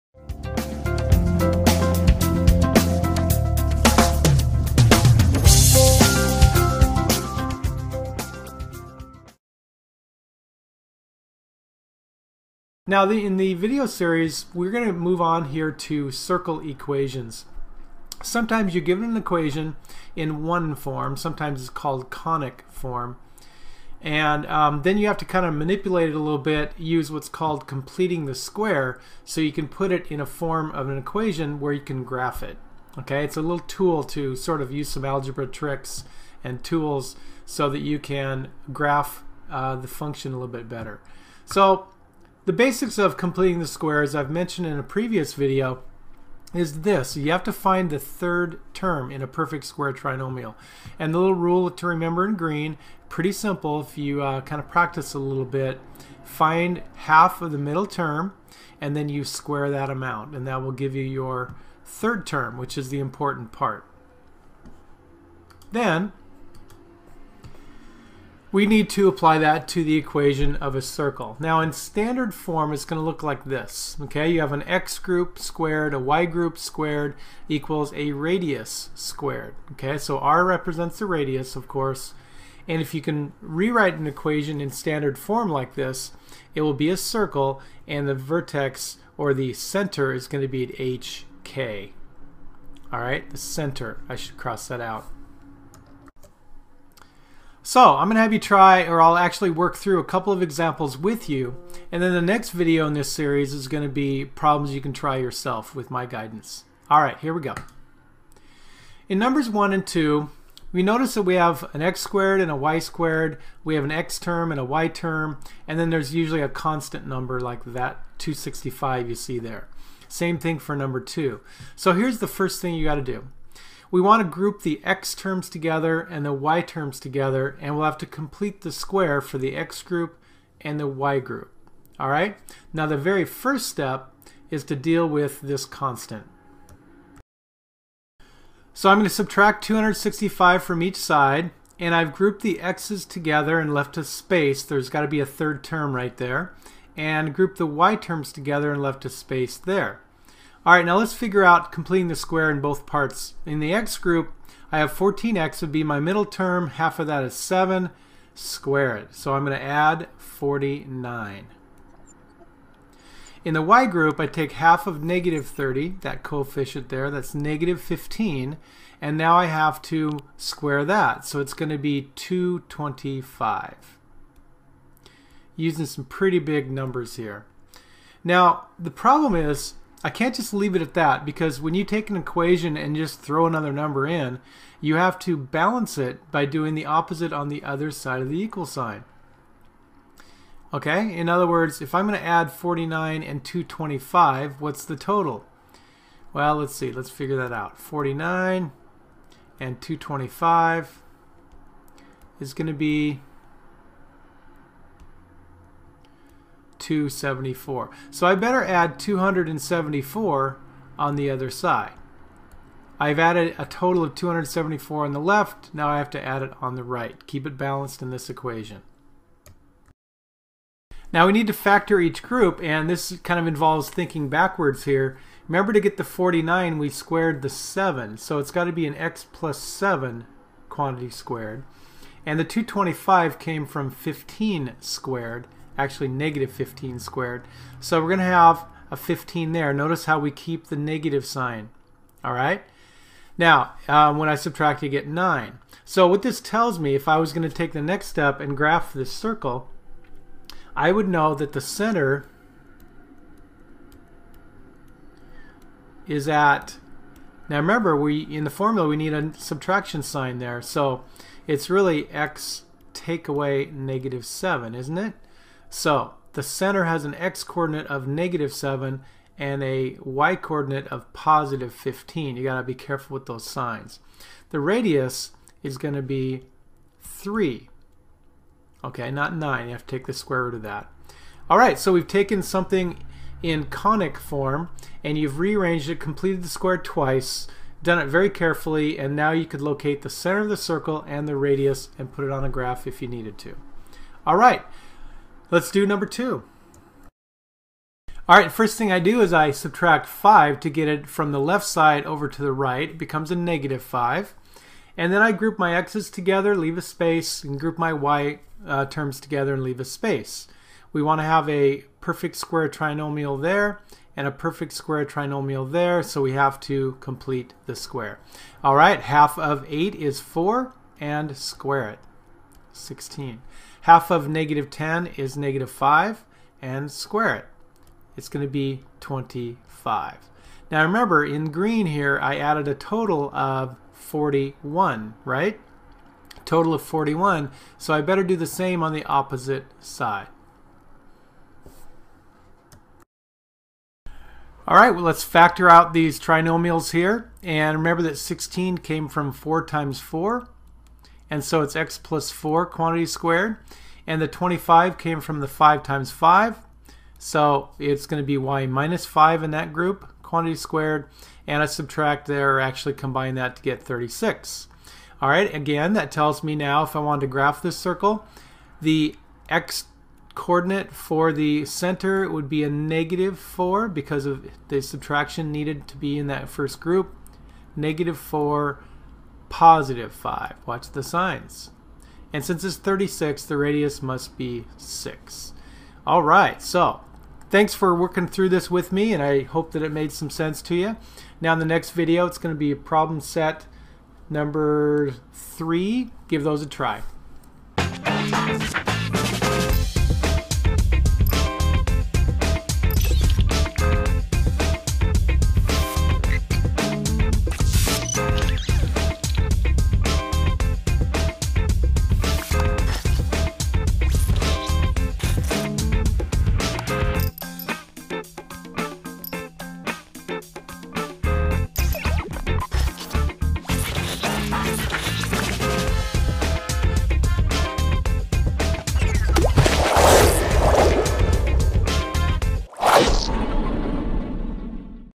Completing the Square to Graph Circles
We use completing the square to rewrite circle equations.  Now we can find the radius and center of the circle to make a graph.  Take a look!